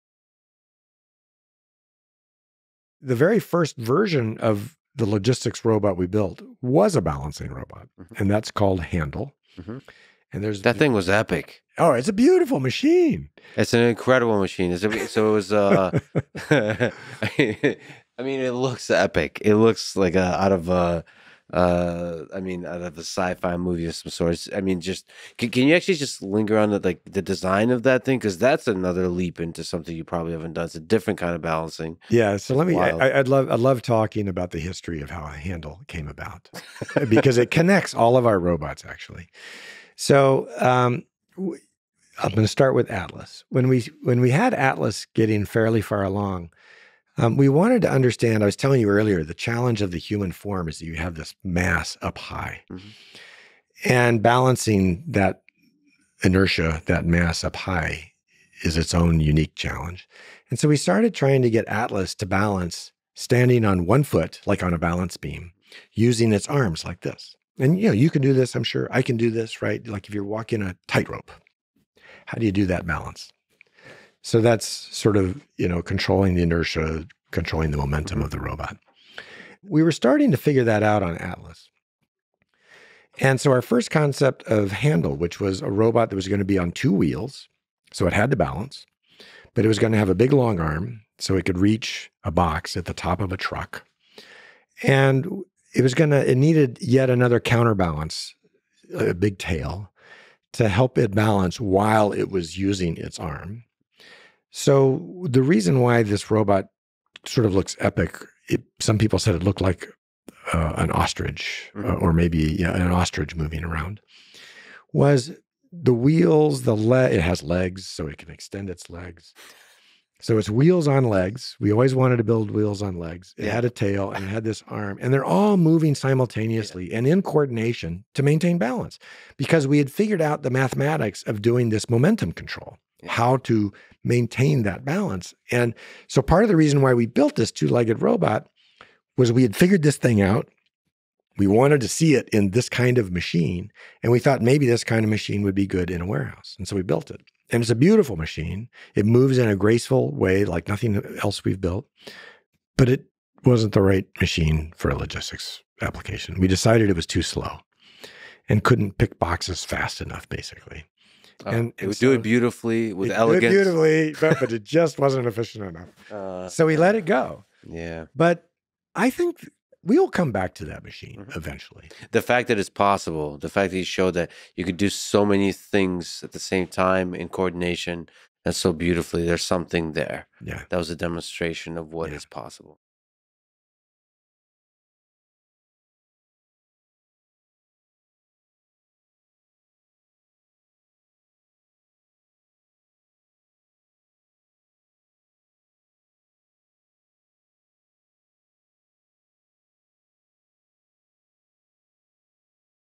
Lex Fridman on the most epic robot he has even seen
GUEST BIO:
Robert Playter is CEO of Boston Dynamics, a legendary robotics company that over 30 years has created some of the most elegant, dextrous, and simply amazing robots ever built, including the humanoid robot Atlas and the robot dog Spot.